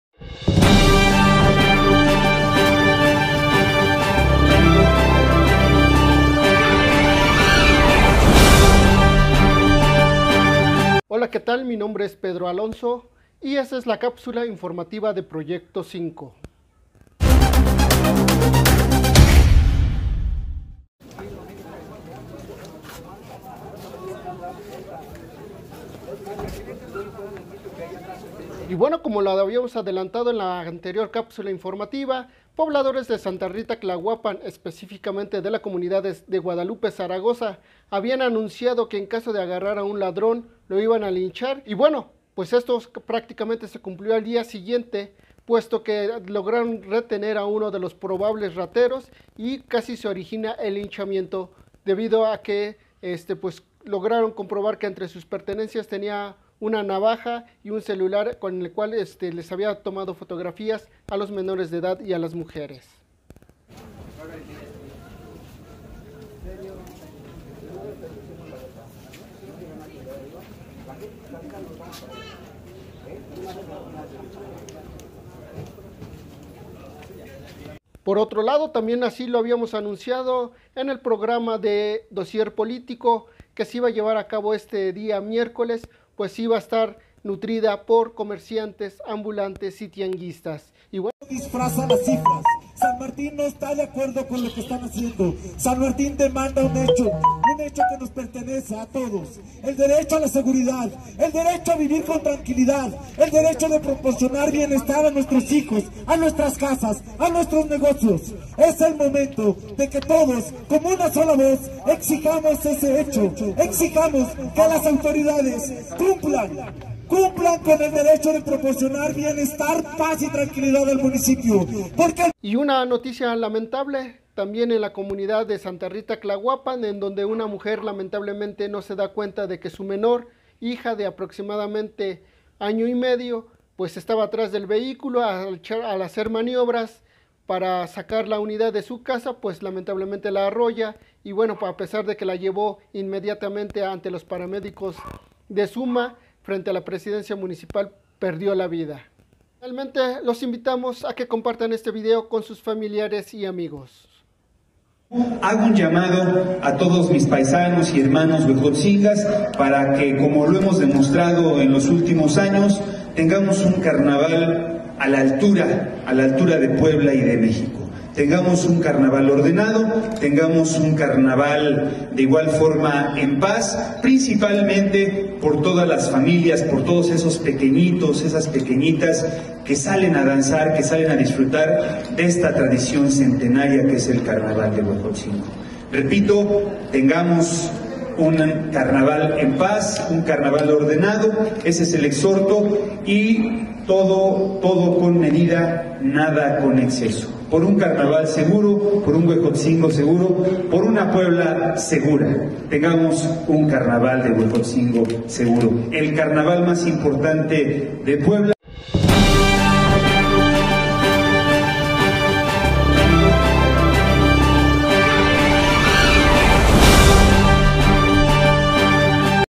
LiveCINCO cápsula informativa Manifestación en Texmelucan y accidente e intento de linchamiento
una manifestación nutrida por ambulantes, tianguistas, y locatarios, nada de ciudadana en San Martín Texmelucan para exigir mayor seguridad. en Guadalupe Zaragoza casi linchan a una persona señalada de ser ladrón. y en Tlahuapan una madre de familia, atropelló a su menor hija.